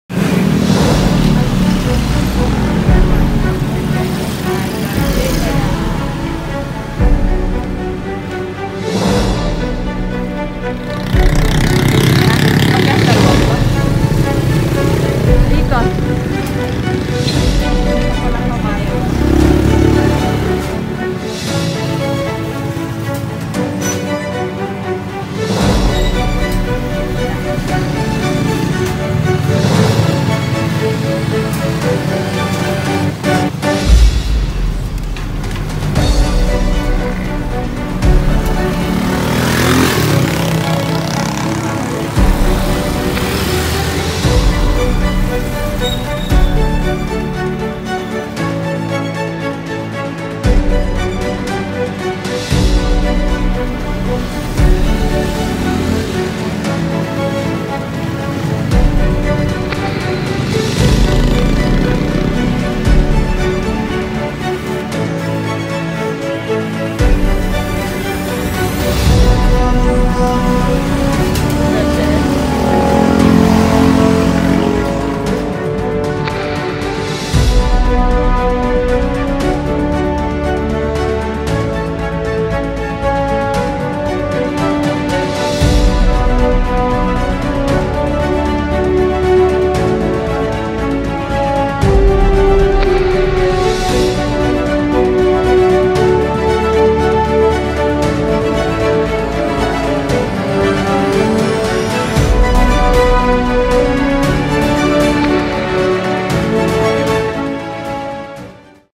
Pinoy breakfast
Lazy to cook breakfast, Pandesal is the go to breakfast. Served with coffee , then breakfast is done.In this Camachiles street, stores line up with different food sold. This panaderia only makes Pandesal, offered original and brushed with butter. We got both. Found a rare commodity, carabao’s milk from the Garcia’s water buffalos. Camachiles used to be a farm( sugarcane,rice, vegetables)but most lands have been developed commercially, but this family still holds on to their farm and farm animals as it was way back then.